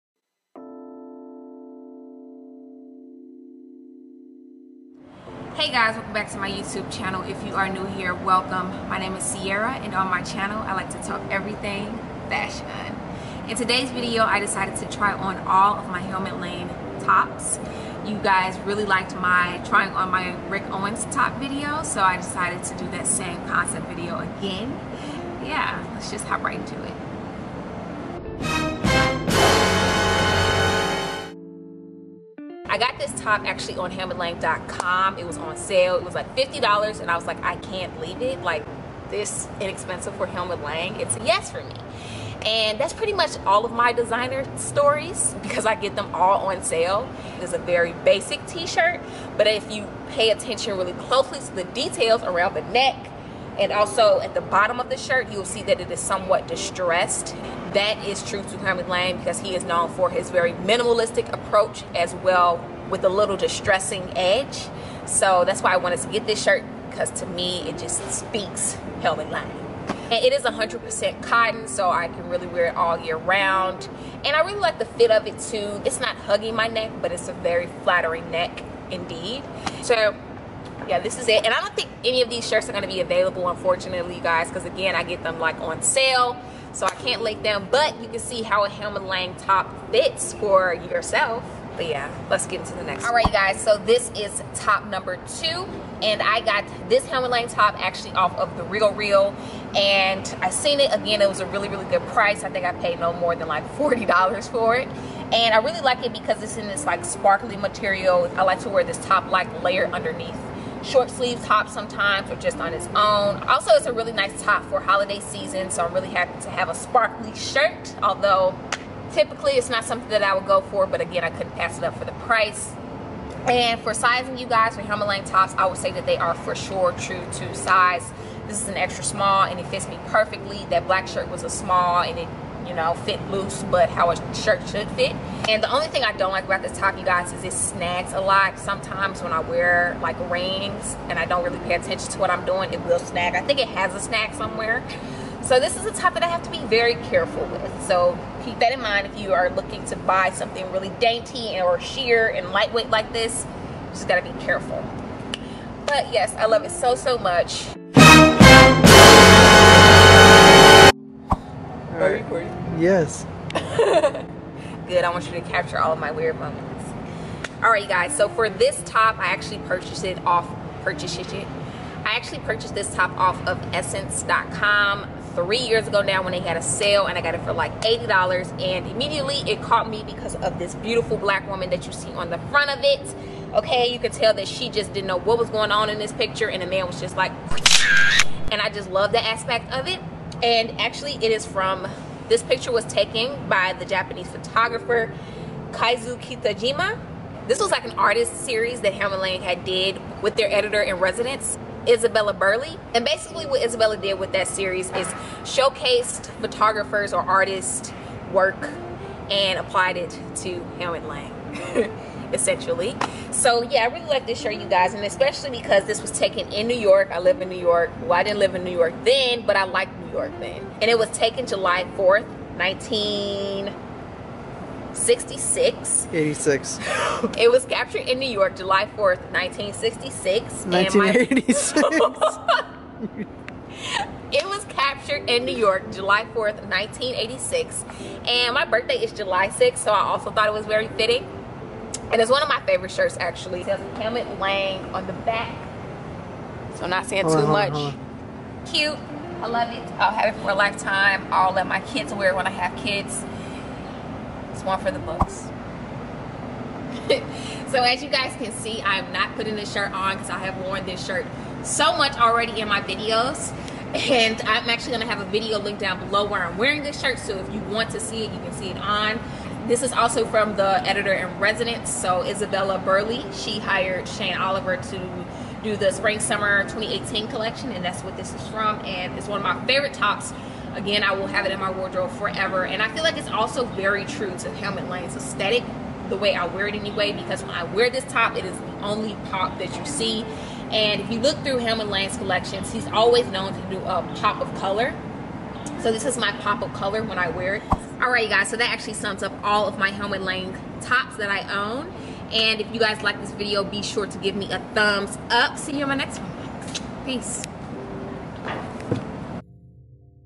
Trying on my Helmut Lang tops
Hey guys,

I decided to try on 3/4 of my Helmut Lang tops. I didn’t try on one because I always wear it and I know you guys are probably tired of seeing it lol! I love the fit of then all, as well as the construction. I hope you guys enjoy this video.

Sorry!

All of these tops aren’t really available anymore, as they are from several season ago.